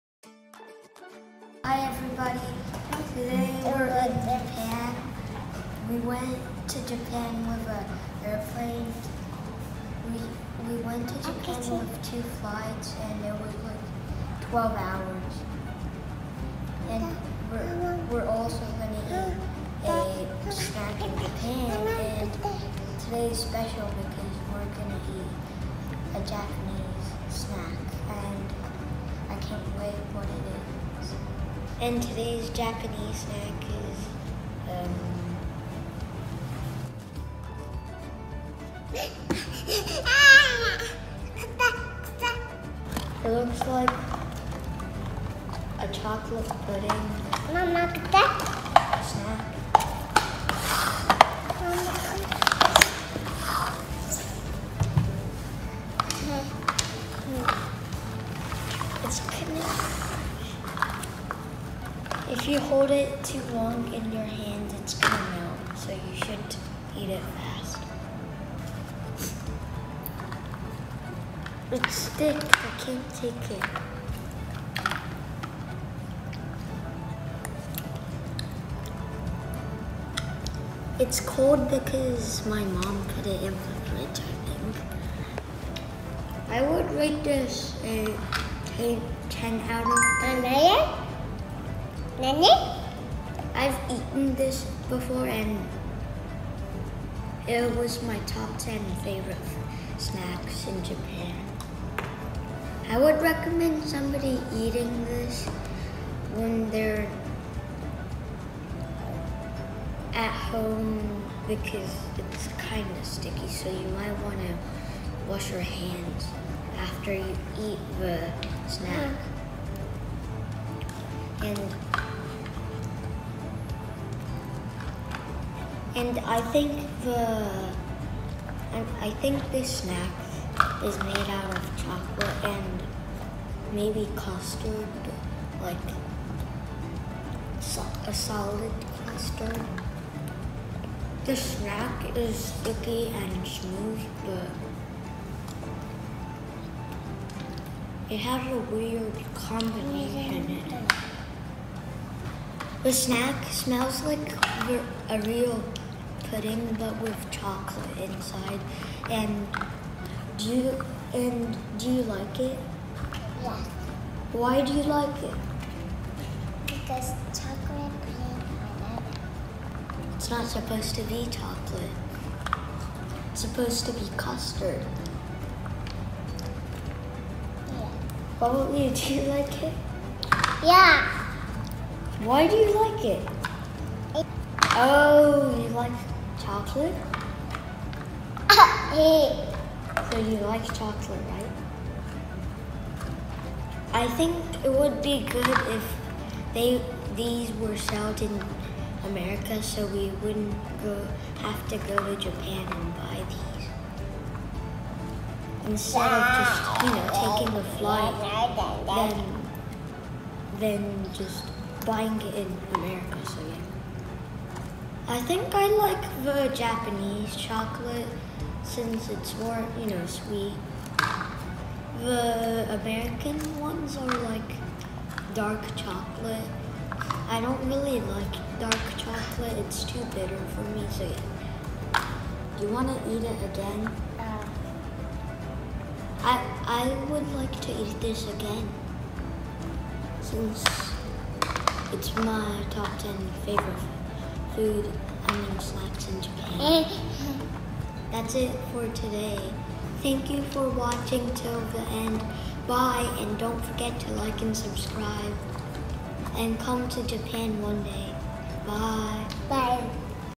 【Japanese Snack】 Trying to eat PUDDING CHOCOLATE
【日本のお菓子】「プリンチョコレート」を食べよう！